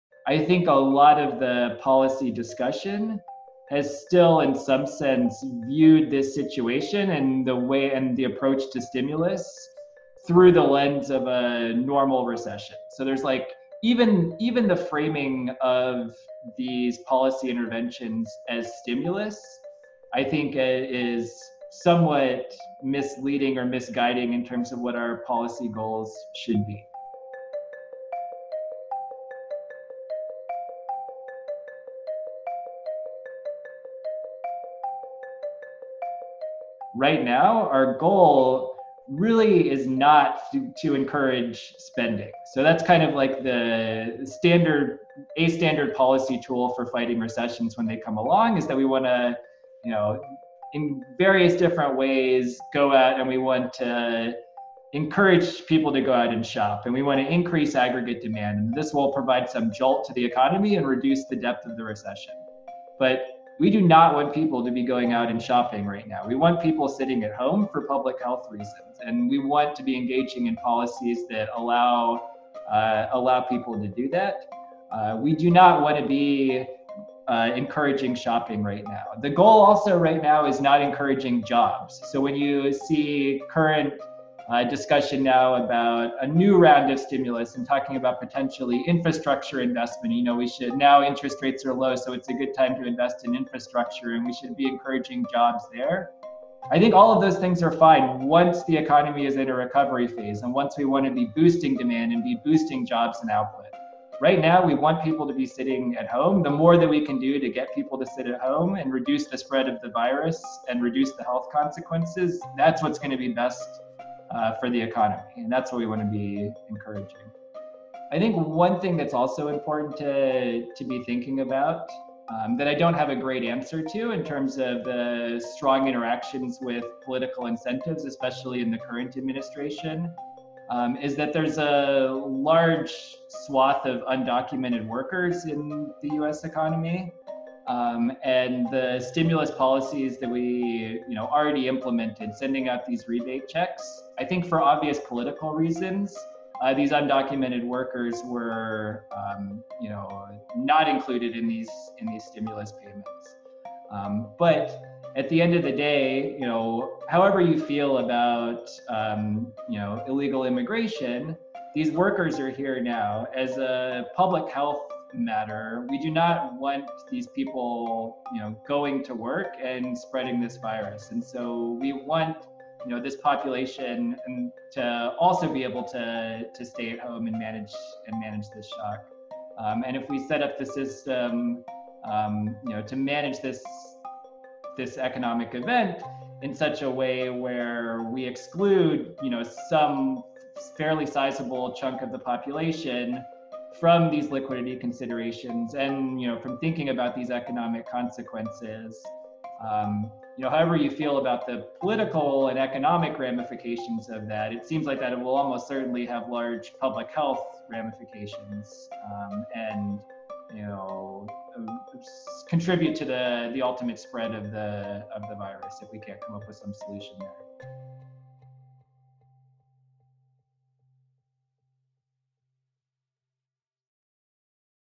Joseph Vavra: Is economic stimulus the right response to the crisis?
review.chicagobooth.edu | When economic calamity hits, the governmental response is often to try to stimulate spending by people and businesses, in order to get money moving through the economy and create new jobs to meet the new demand. But Chicago Booth’s Joseph S. Vavra says that our policy goals now are different than they usually are in a recession, and policy makers should be considering how to tailor their response to this crisis to suit the unusual circumstances.